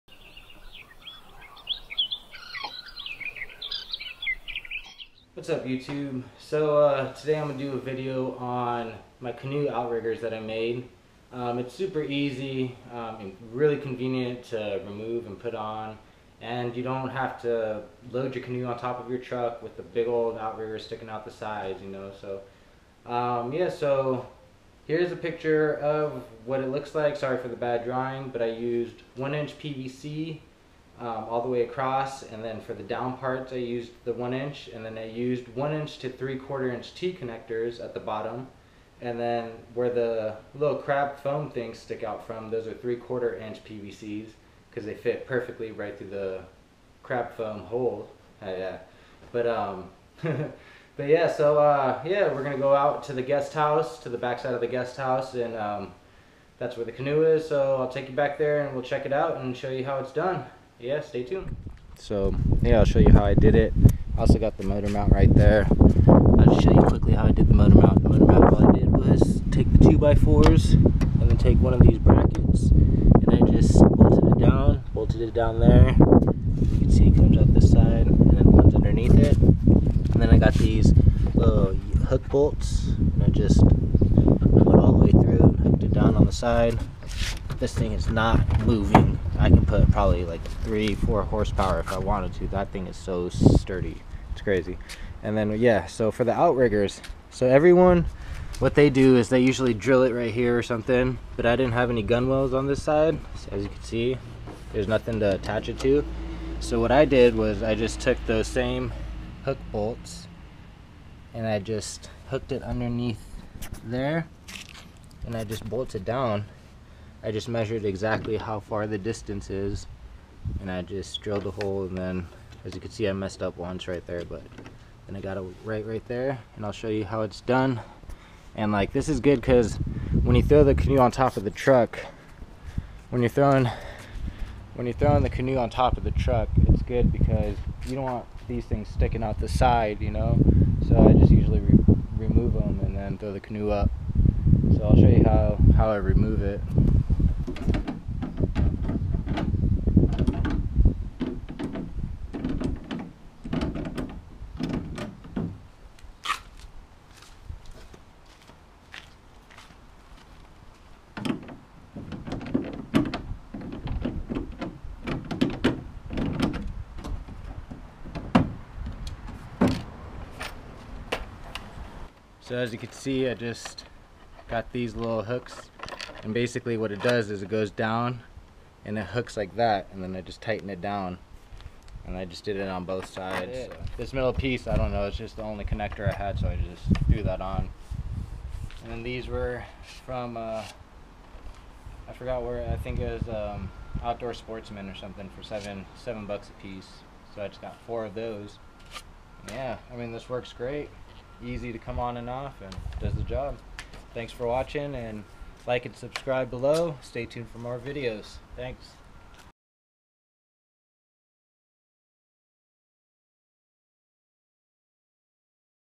DIY Canoe Outrigger/stabilizers Homemade
Here is a video of some canoe outriggers i made... I noticed everyone was drilling theirs down into the canoe. So i decide to go with a little different approach. These outriggers make fishing from the canoe SUPER stable and you can stand and fish! Hope you like the video!